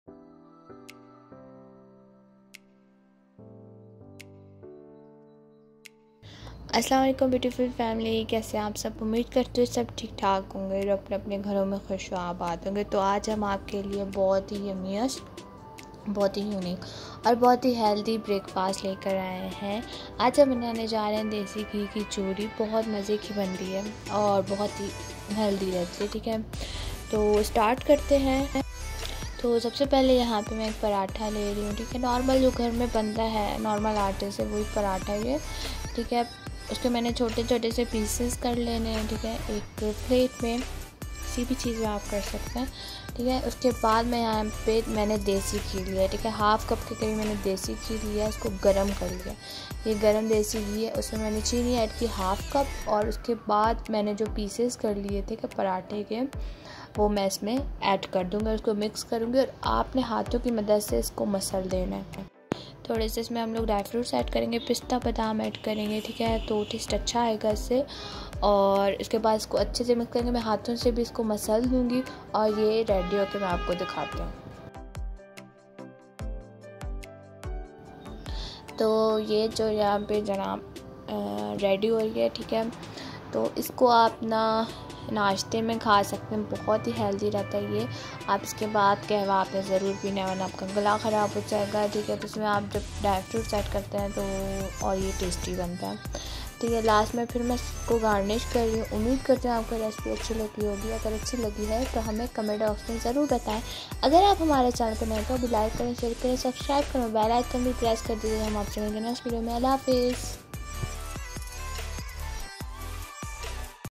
Healthy Breakfast Desi Ghee Ki Chori | Breakfast Ideas | Girls healthy Journey
RECIPE :
Desi Ghee
Paratha
Suger
Dry Fruits

Chapters :
0:00 Introduction
0:28 Making pieces of paratha
0:46 Making Desi ghee mixture
1:33 Adding Dry Fruits
1:50 Outro